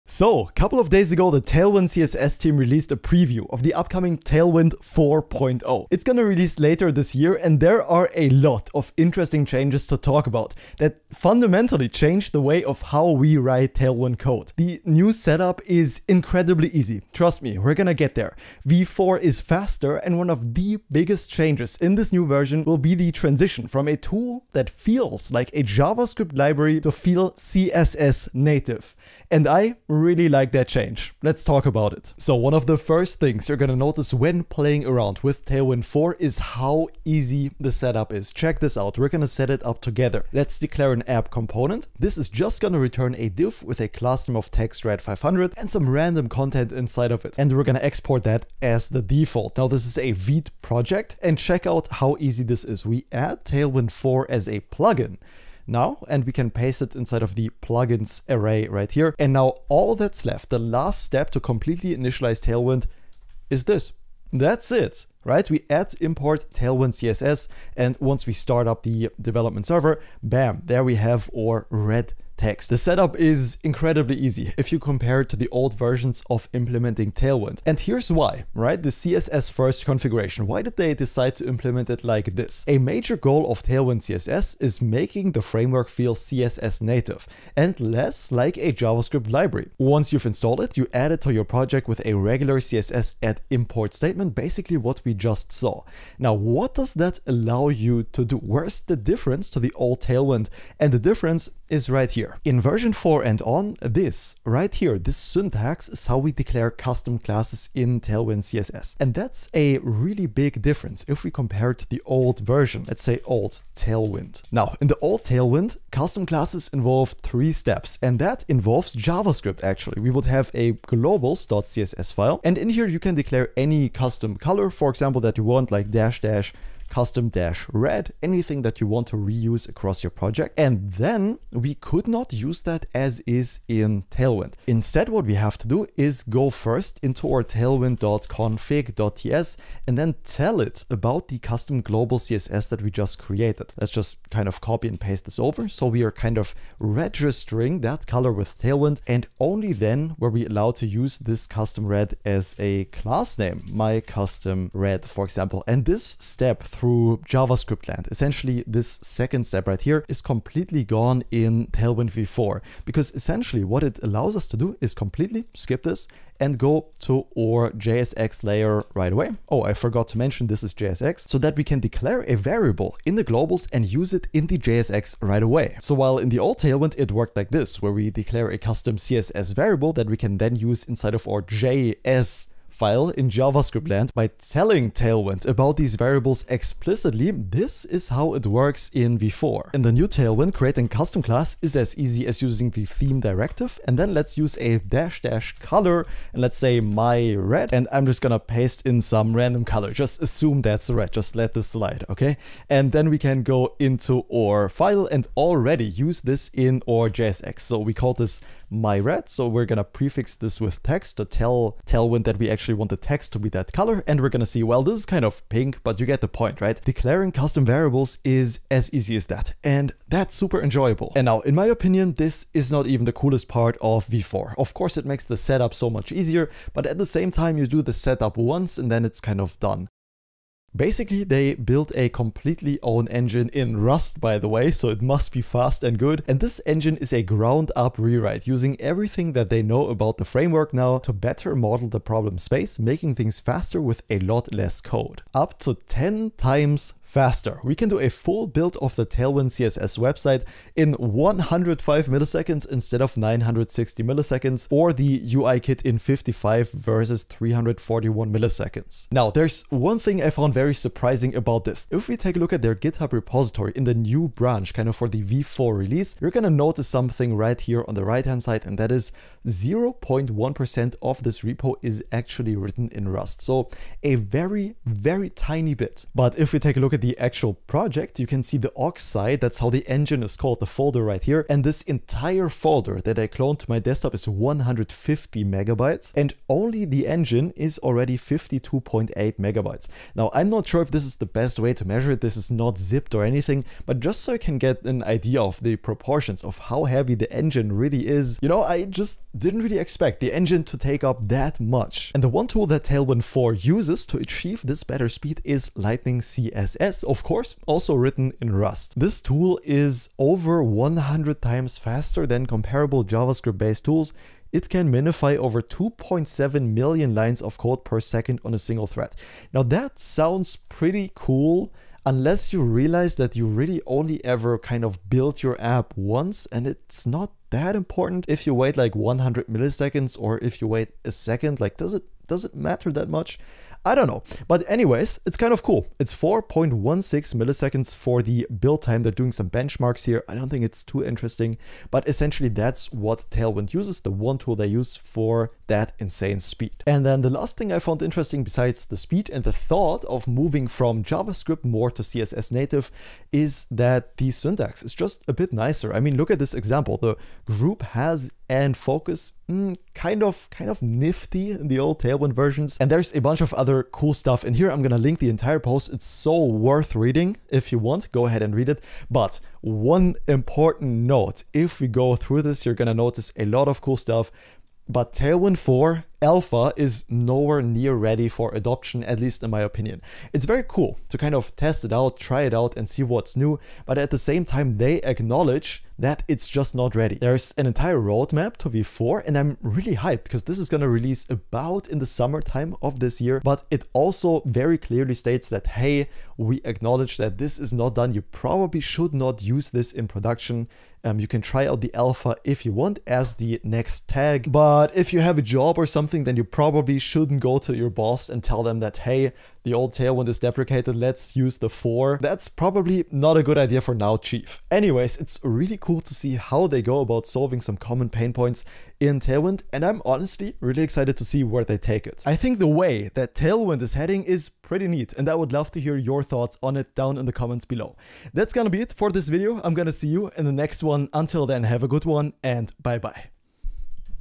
The New TailwindCSS Looks Promising
A few days ago, tailwindcss released their 4.0 preview. It's already available to play around with, so let's see what changed and how it tries to be better than previous versions.